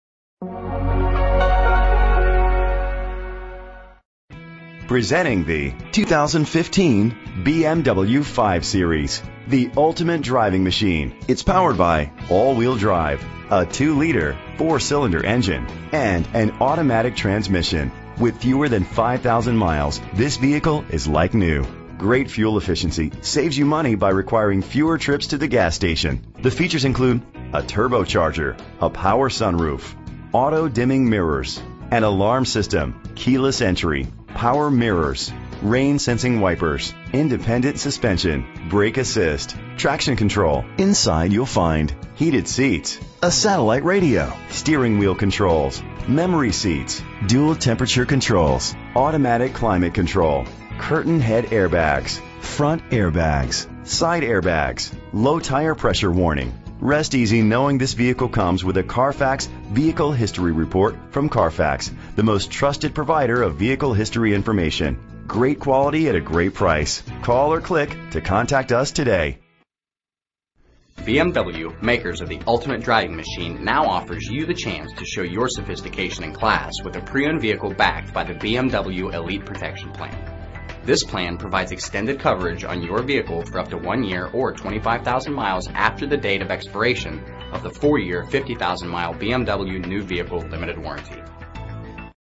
Circle BMW - 2015 BMW 5 Series - Eatontown, NJ
Year: 2015
 Make: BMW
 Model: 5 Series
 Trim: 528i xDrive
 Engine: 2.0 liter 4 Cylindercylinder AWD
 Transmission: 8-Speed
 Color: Dk. Blue
 Mileage: 4341
 Address: